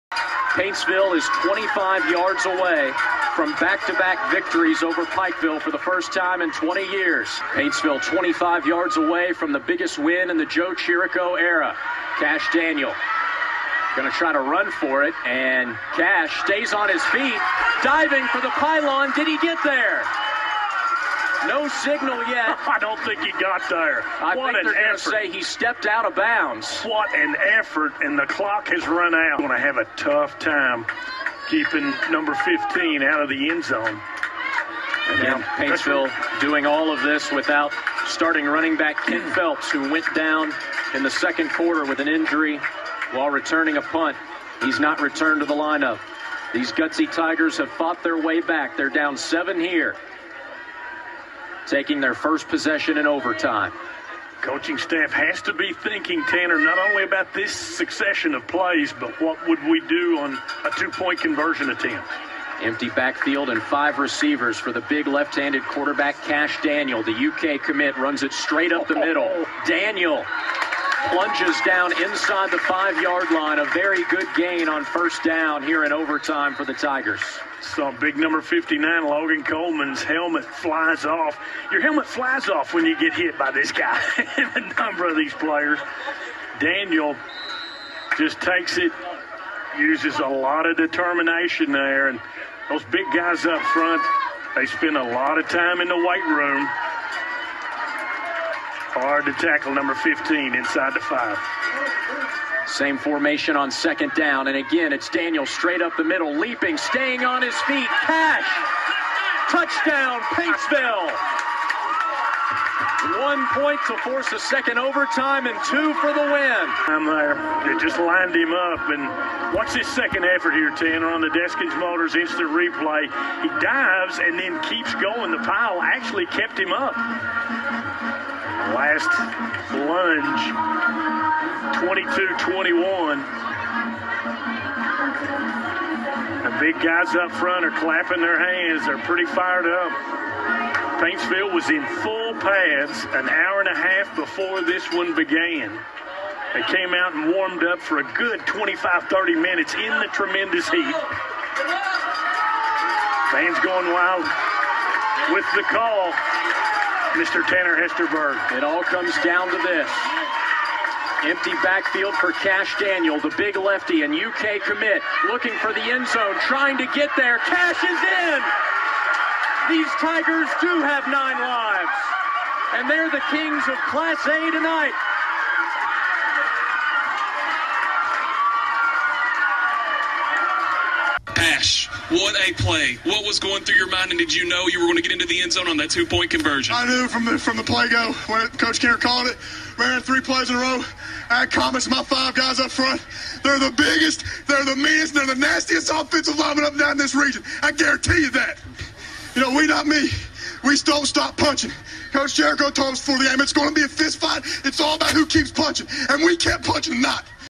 Pikeville Week - Kash Daniel burst on to scene 2015!
In honor of Pikeville week - The moment Kash Daniel burst on to the scene in the 2015 OT thriller!